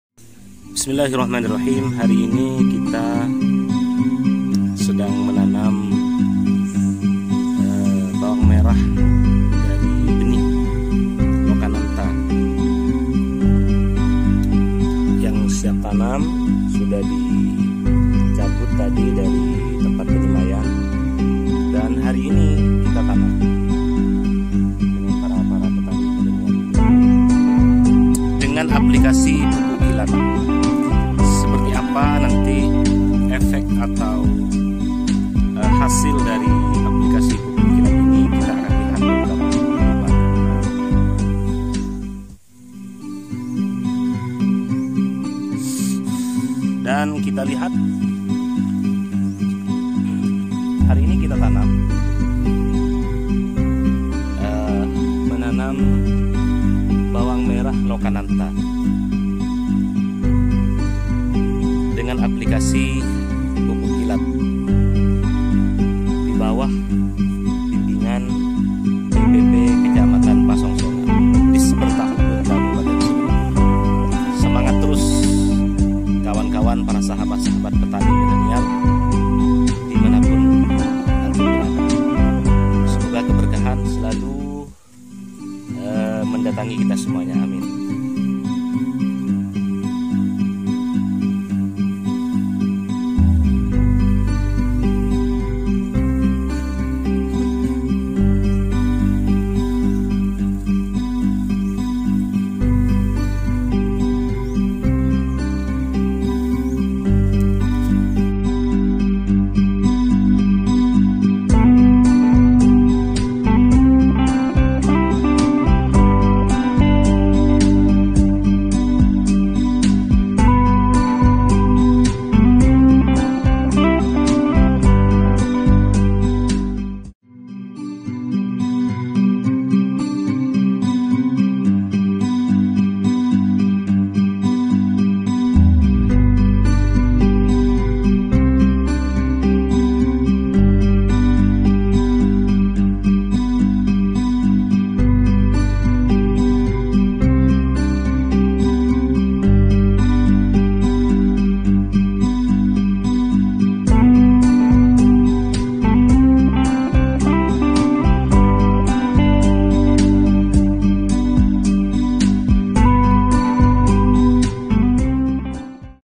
Cara Menanam Bawang Merah
Cara Menanam Bawang Merah aplikasi Pupuk Kilat pada Bawang Merah dari Benih Lokananta yang sudah siap tanam di Wilayah Pasongsongan Kab. Sumenep Madura, dibawah bimbingan BPP Kecamatan Pasongsongan Dispertahortbun Kabupaten Sumenep

Semoga Sukses Panen Melimpah__